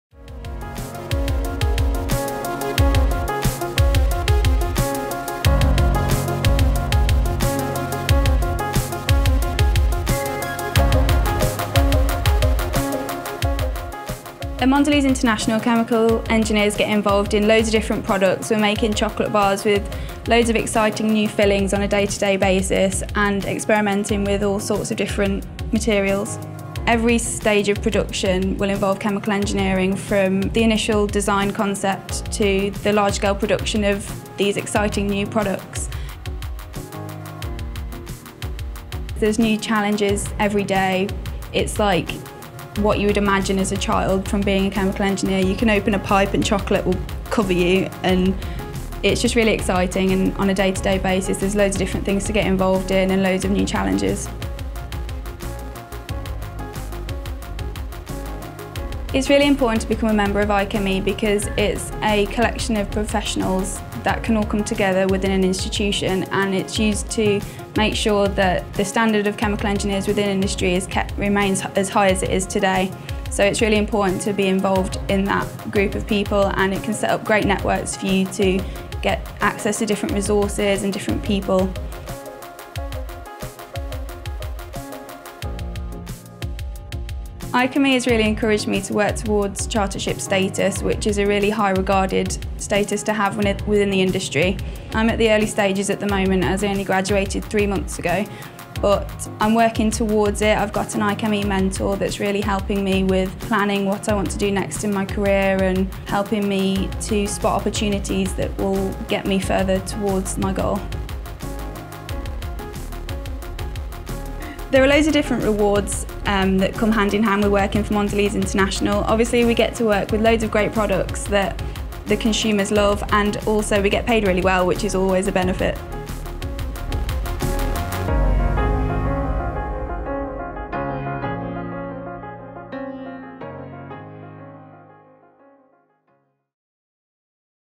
ChemEngProfiles - Mondelez's Bryony Robinson
Bryony Robinson, graduate trainee at Mondelez's Bournville factory, talks about her job in global chocolate research, design and production.